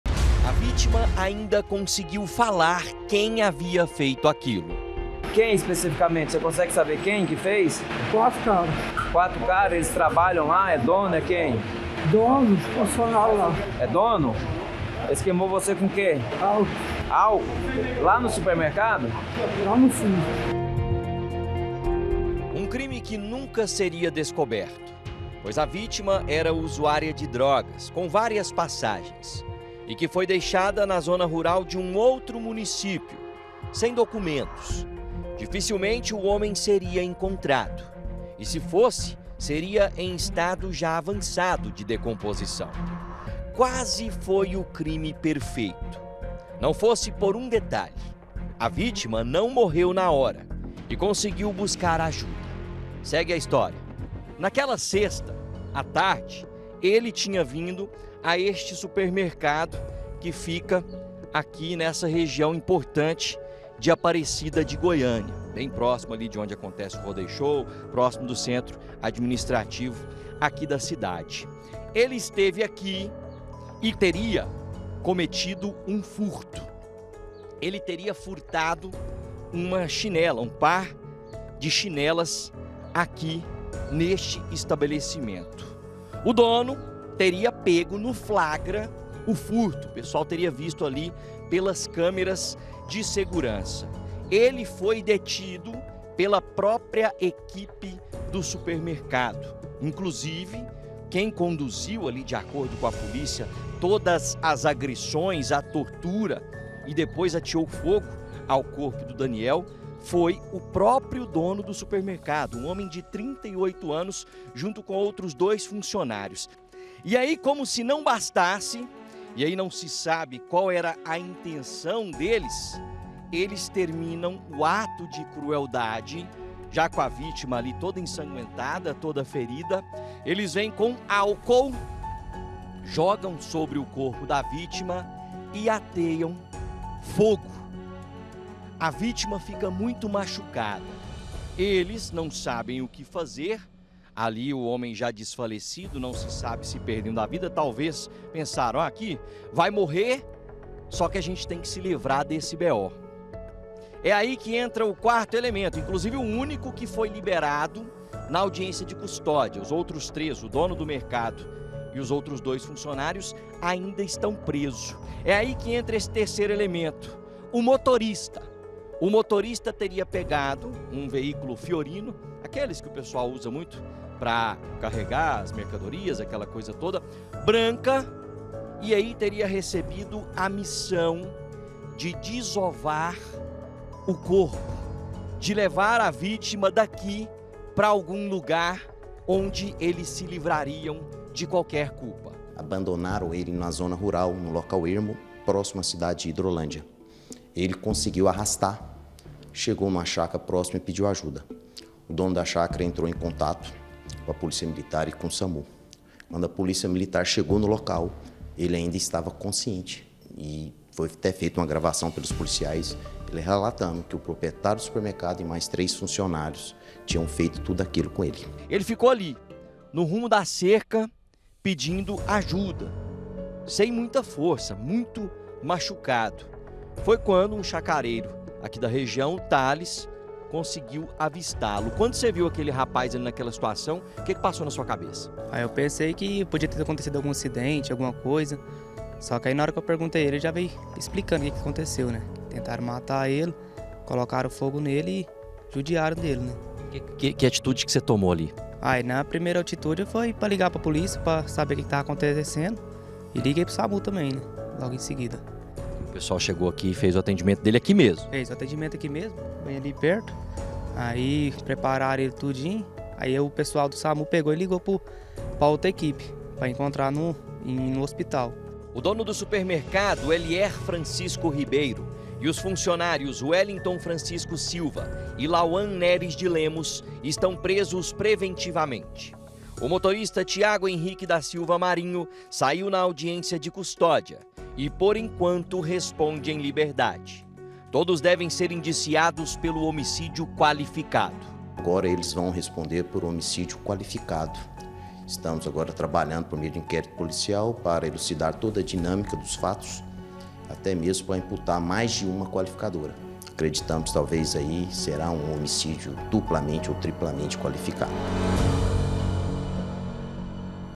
DONO E FUNCIONÁRIOS PRESOS
O HOMEM QUE TEVE OITENTA POR CENTO DO CORPO QUEIMADO, DEPOIS DE SER ESPANCADO DENTRO DE UM SUPERMERCADO EM APARECIDA, NÃO RESISTIU AOS FERIMENTOS. TUDO PORQUE A VÍTIMA TERIA FURTADO SANDÁLIAS. TRÊS SUSPEITOS DO CRIME CONTINUAM PRESOS.